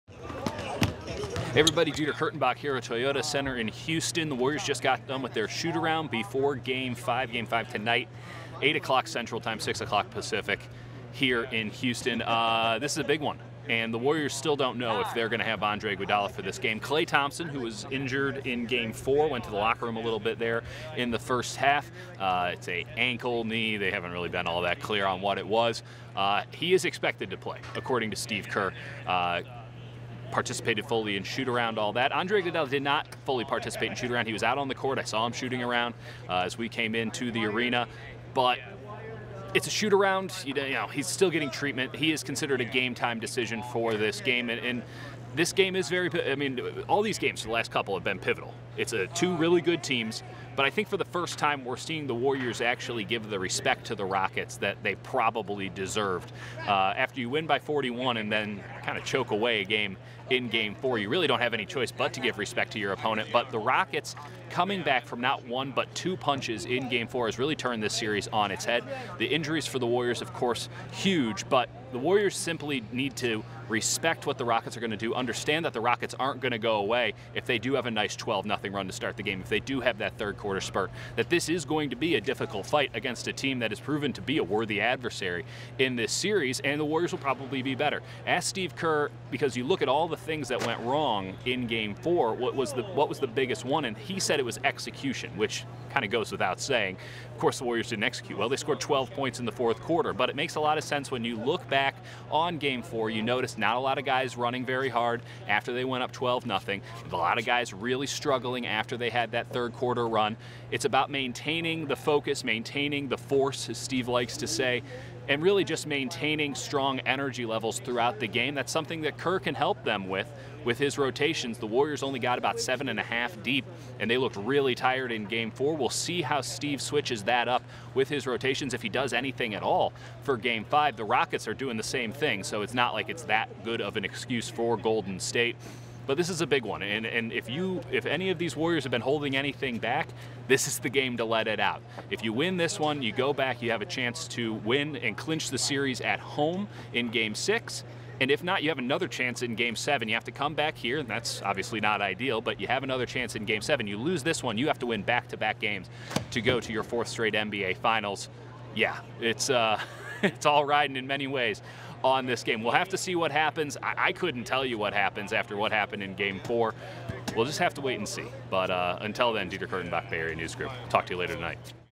Dieter Kurtenbach previews Game 5 of the Western Conference Finals
Dieter Kurtenbach previews Game 5 of the Western Conference Finals between the Golden State Warriors and the Houston Rockets.